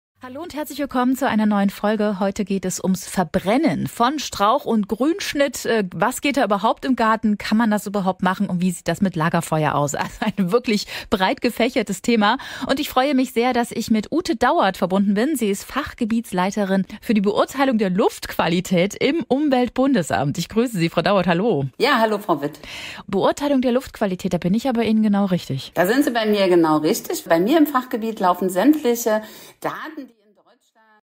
"Verbrennung im Garten: Tipps zur Luftqualität & Umweltbewusstsein"
In der aktuellen Episode des MDR Garten-Podcasts tauchen wir ein in die Welt des Verbrennens von Strauchschnitt und Lagerfeuern im Garten, begleitet von spannenden Einblicken der Luftqualitätsexpertin Ute Dauert vom Umweltbundesamt. Hören Sie diese erhellenden 20 Minuten, während Sie Unkraut aus dem Garten jäten und Ihre grüne Oase vorbereiten!